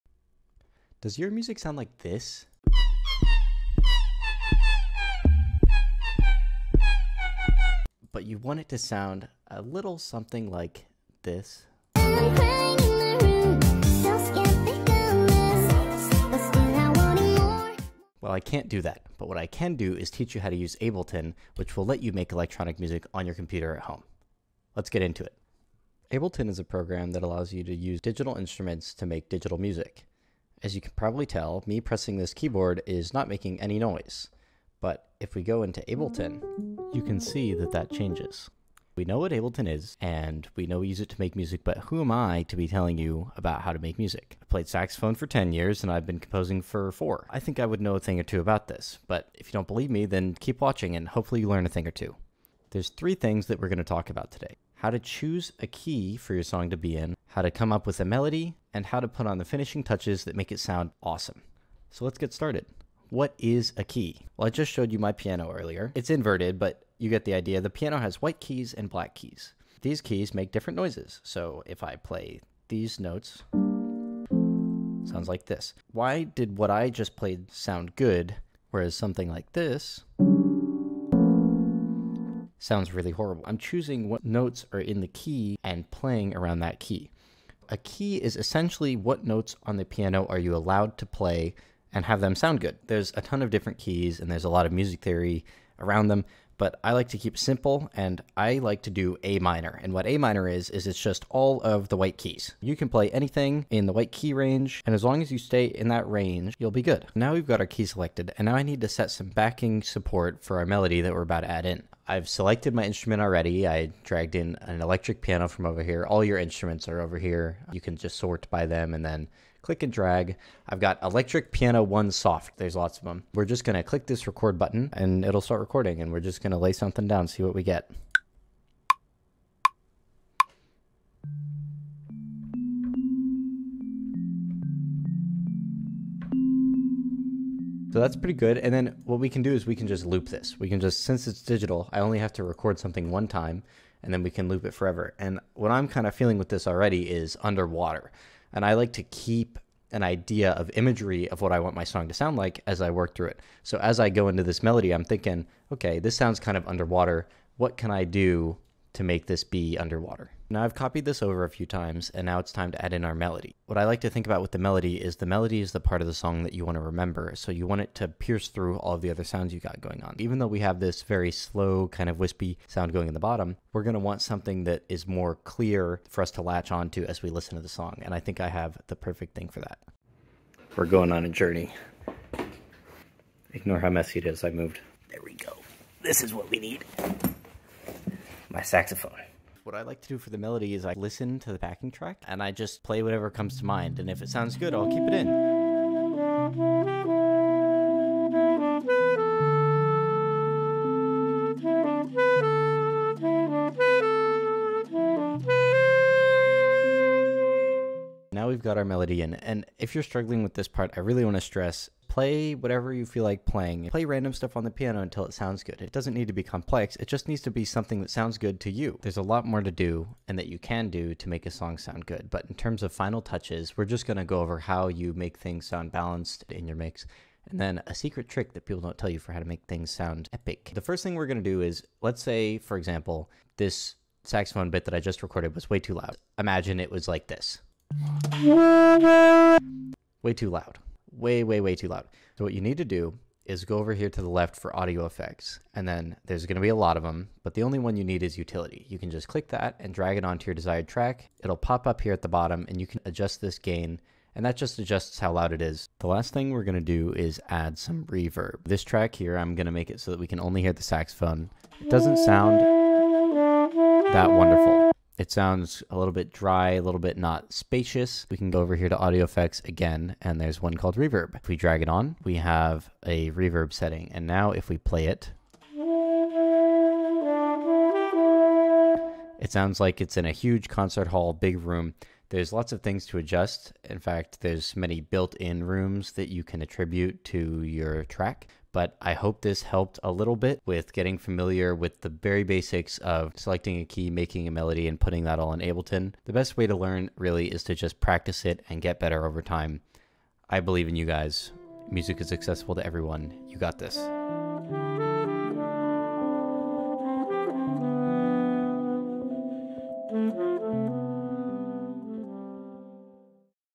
How to make a song in Ableton!
Geared towards people with little to no experience about music, I talk about the basics of selecting a key, formulating a melody, and applying final audio effects to your audio to help you transform and idea into a full song.

Since I didn't have time to include the song I made for this video in it, here's the song